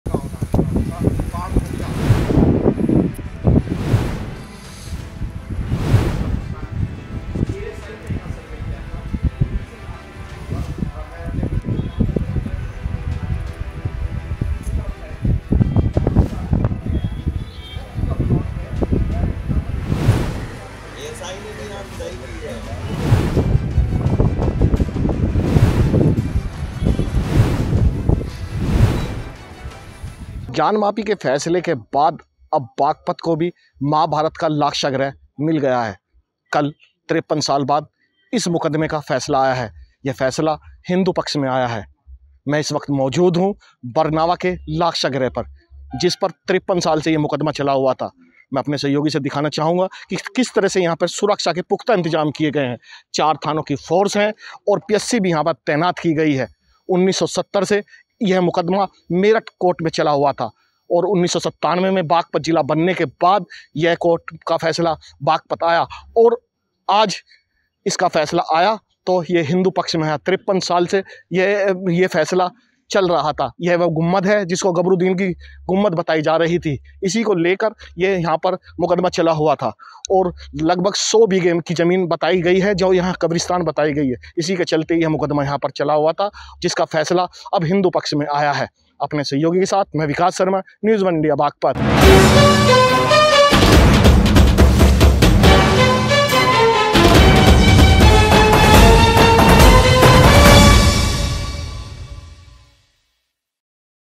Baghpat Lakshagrah News : बागपत लाक्षागृह के फैसले के बाद सुरक्षा को लेकर पुलिस का अलर्ट Lakshagraha
Security arrangements increased after the decision on Lakshagriha Heavy police force and PAC deployed for the security of Lakshagriha Police keeping a close watch on anti-social elements Lakshagraha mound located in Barnawa and There was a dispute regarding the tomb Baghpat court had given the verdict in favor of Hindus Lakshagriha mound of Mahabharata period is situated in Barnawa village of Baghpat.

लाक्षागृह पर फैसला आने के बाद बढ़ाई गई सुरक्षा व्यवस्था लाक्षागृह की सुरक्षा में भारी पुलिस बल और पीएसी तैनात असामाजिक तत्वों पर पुलिस की पैनी नजर बरनावा स्थित लाक्षाग्रह टीले और मजार को लेकर था विवाद बागपत कोर्ट ने हिंदू पक्ष में सुनाया था फैसला बागपत के बरनावा गांव स्थित है महाभारत कालीन लाक्षागृह टीला